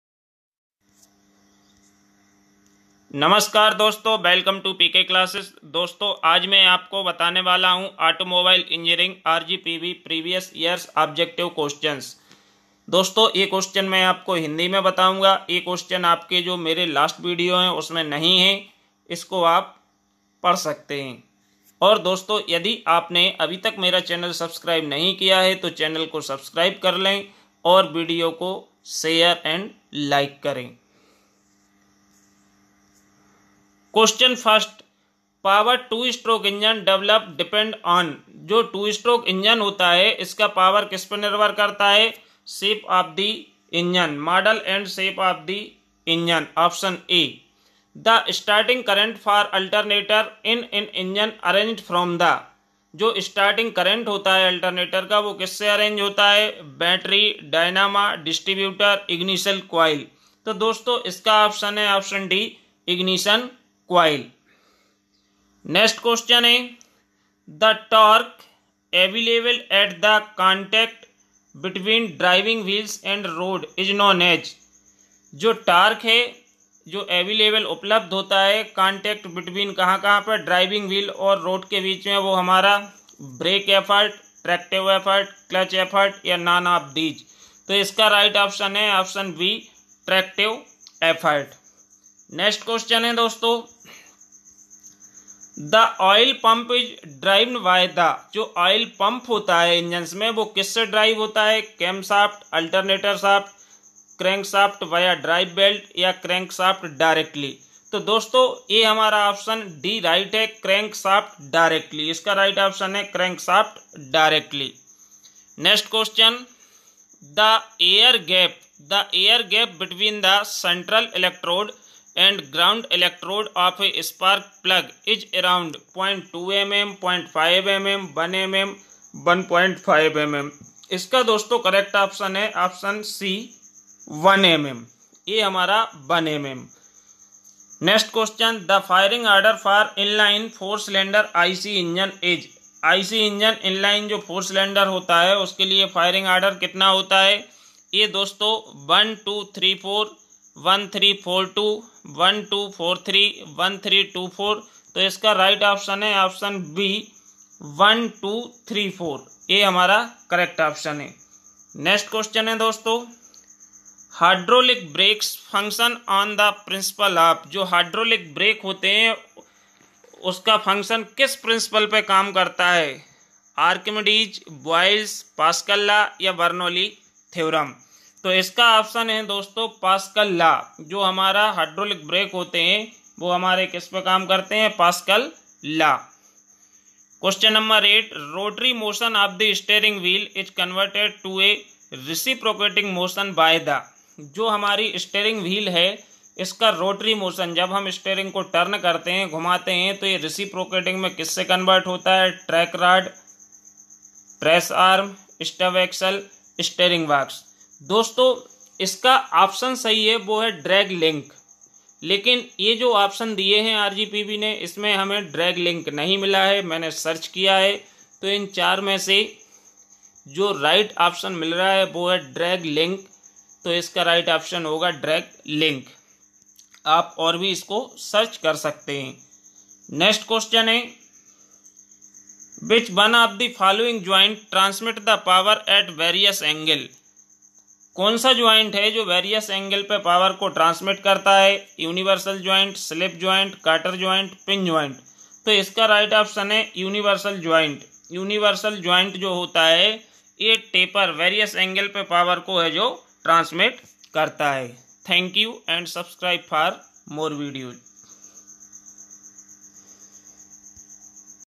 Automobiles engineering objective questions
Automobiles engineering RGPV previous years objective questions and answers